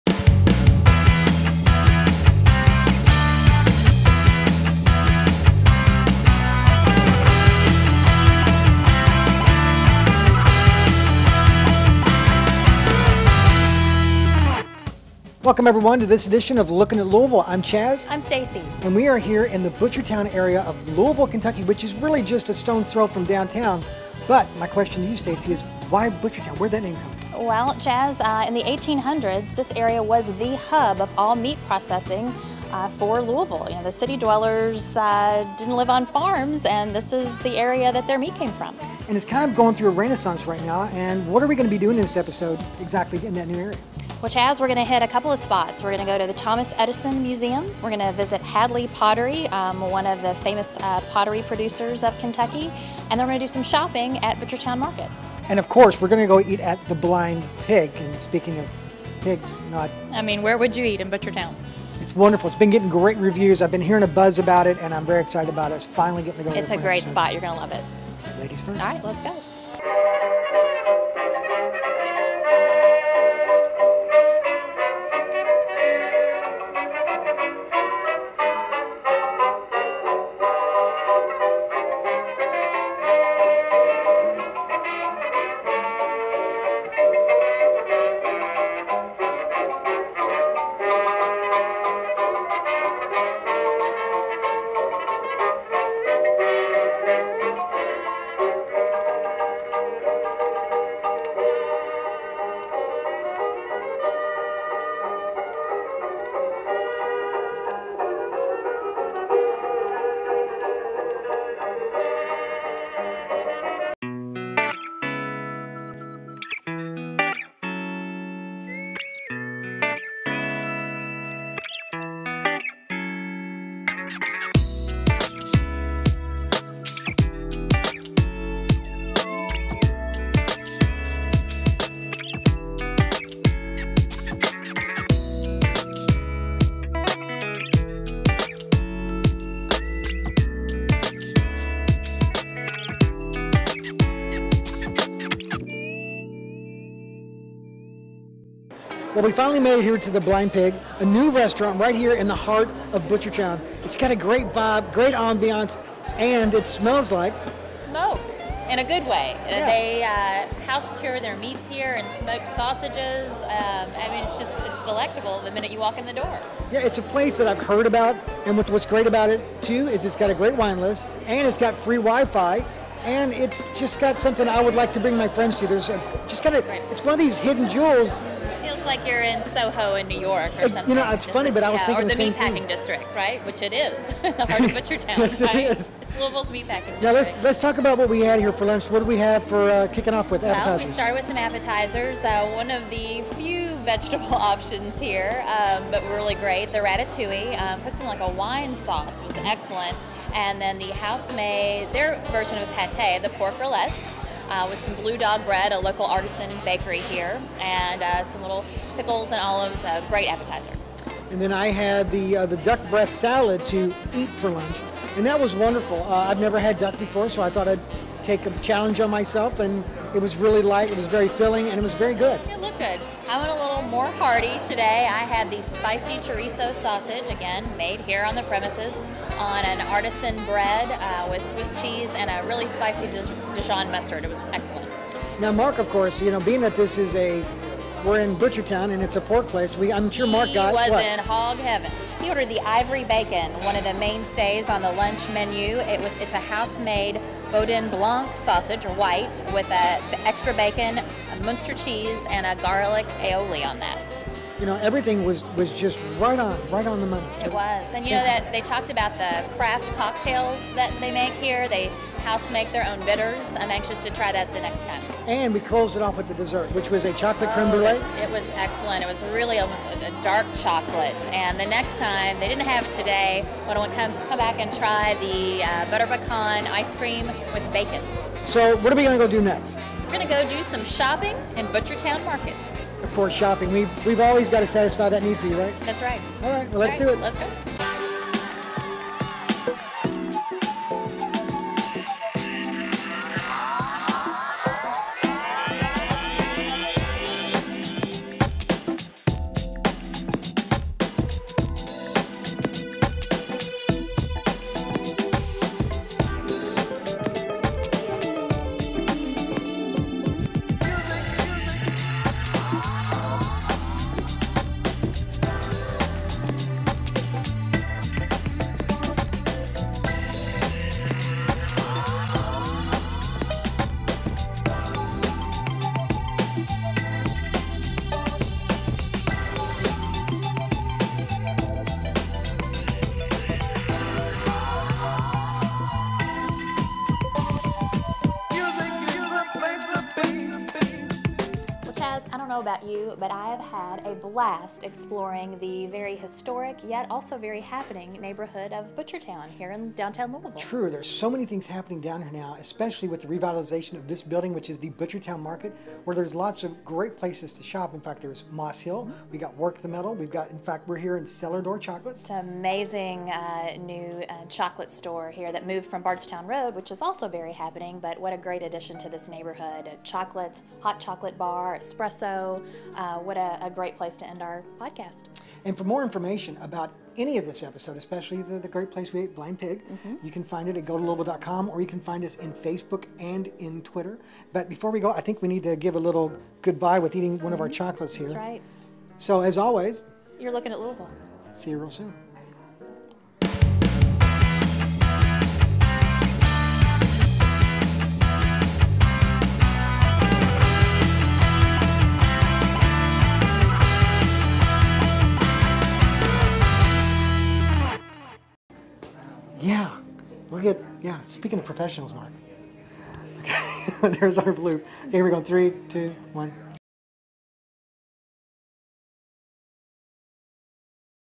Exploring Butchertown
It's Louisville's Meat-Packing District. See the hard-working Victorian era neighborhood of Butchertown come back to life with vibrant boutiques, time-honored pottery and unique attractions like the Thomas Edison House.  Dining at Blind Pig and Cellar Door Chocolates.